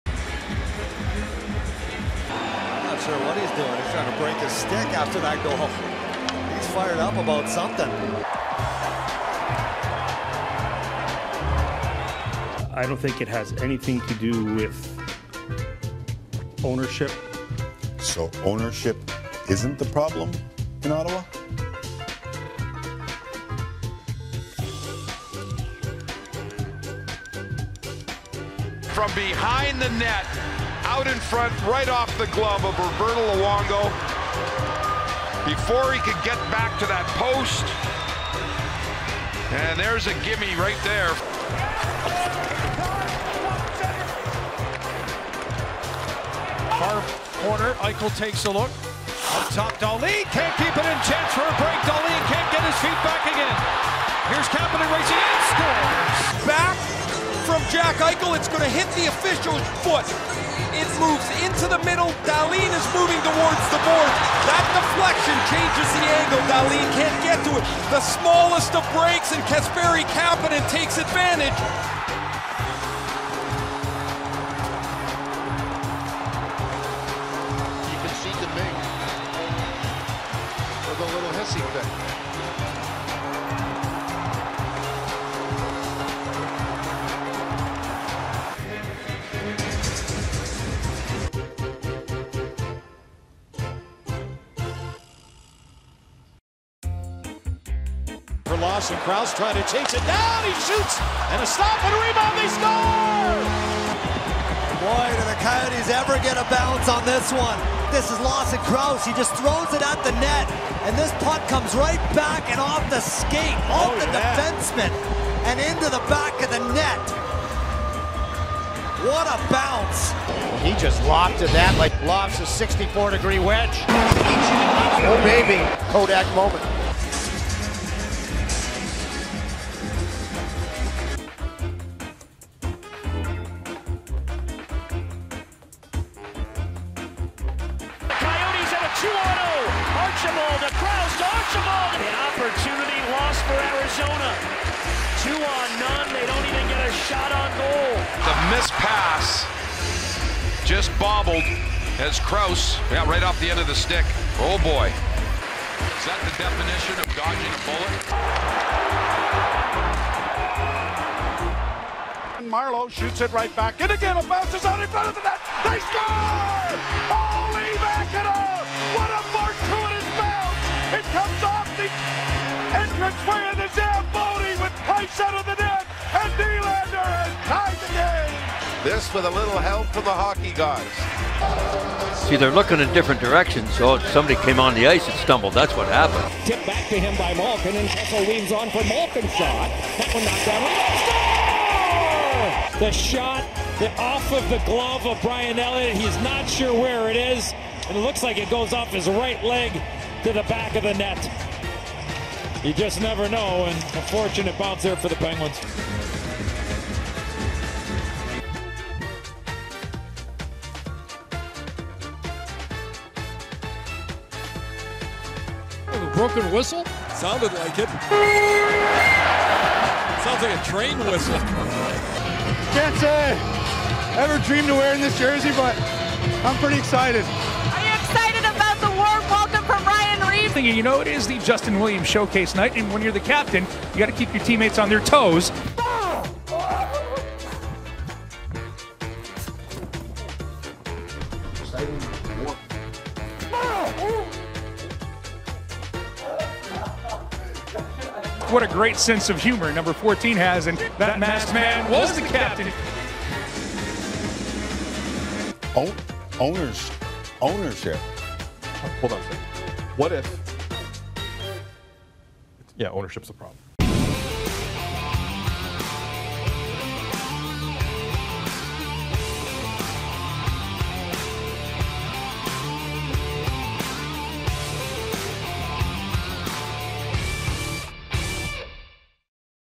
NHL Bloopers of The Week: How Do You Screw Up A 2 on 0!?
NHL Bloopers of the week is here featuring Arizona missing a 2 on 0 break, Tim and Sid up to the same old shenanigans and Carolina missing the net by about 15 feet? Let the fun begin!